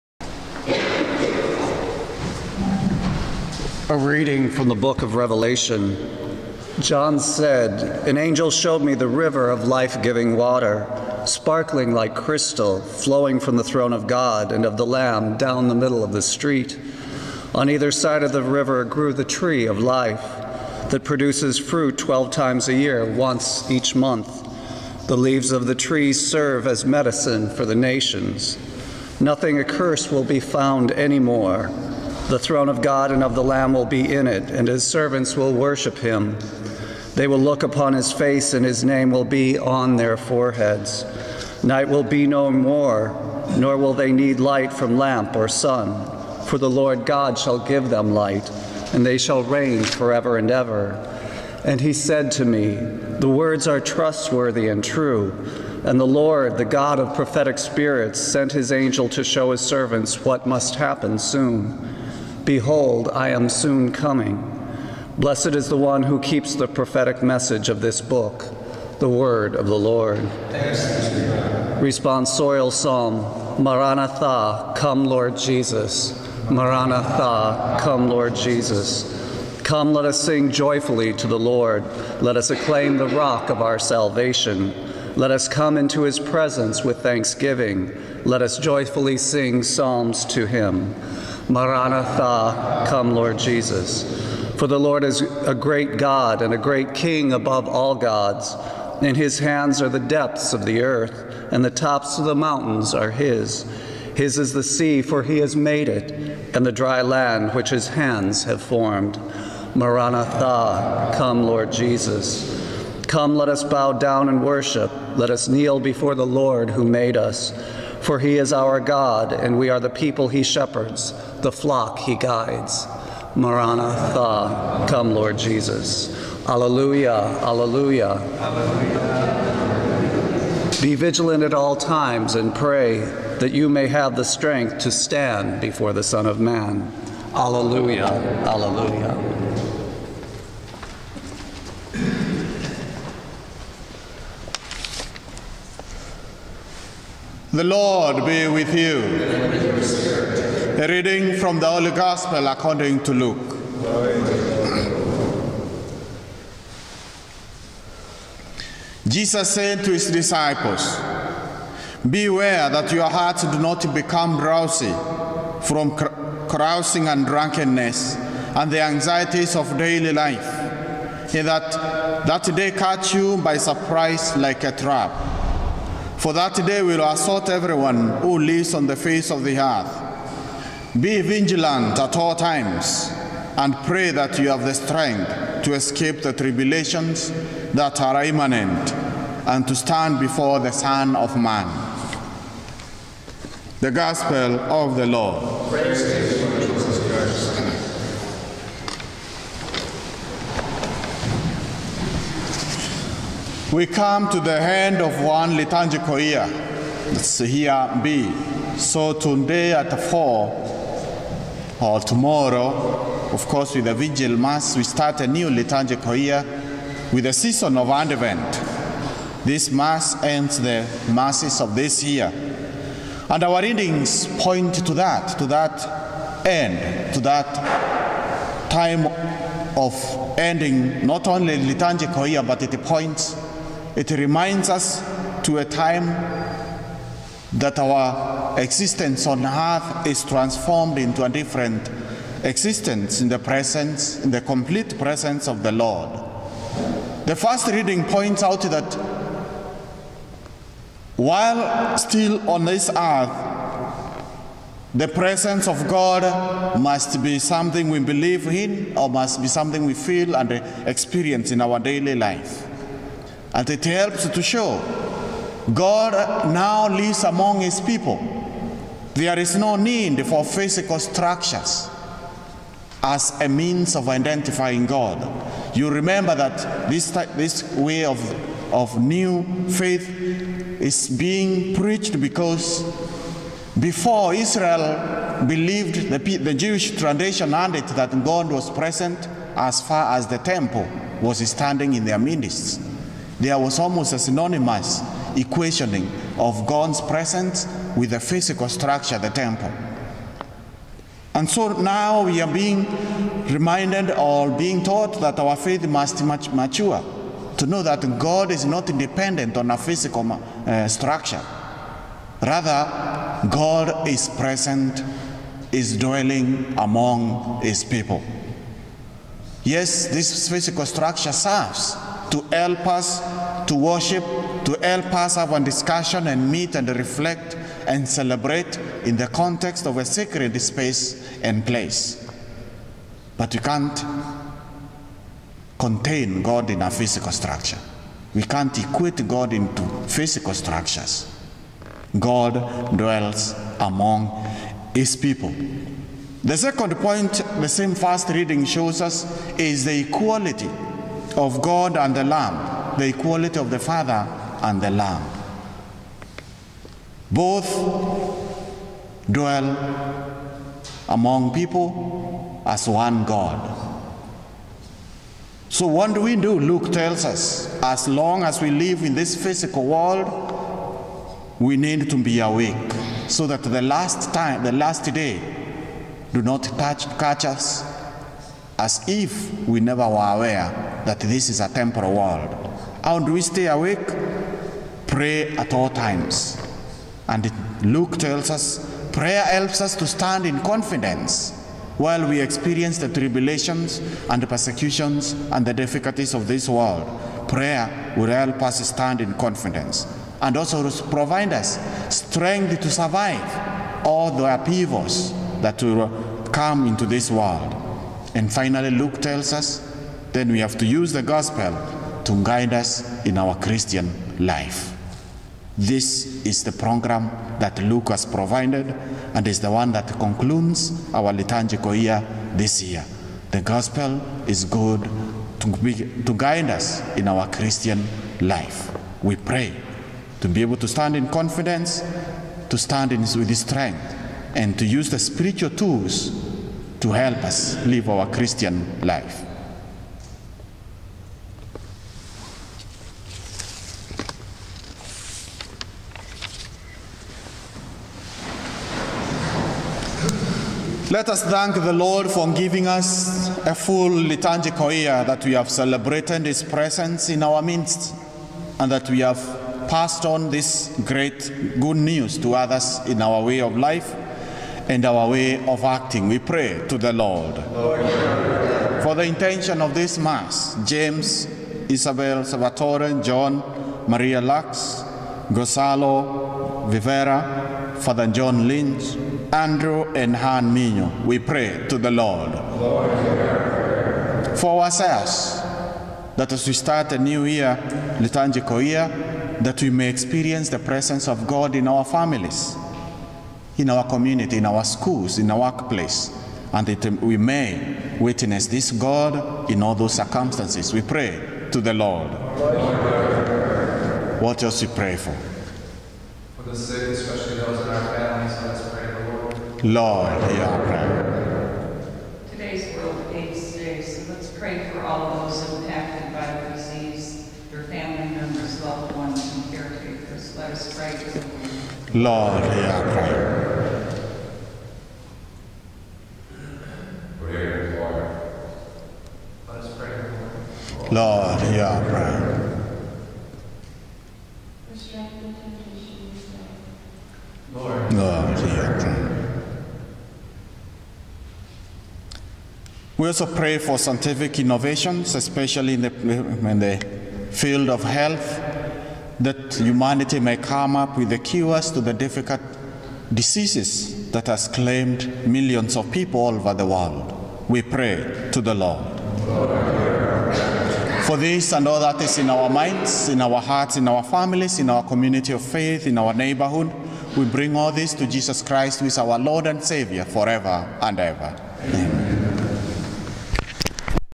Thirty-Fouth Week Ordinary Time, Santuday Year II. Daily Reflection With Fr. Kinoti
Start every day at St. Ignatius with readings from the old and new testament followed by an inspirational reflection with Fr. Kinoti.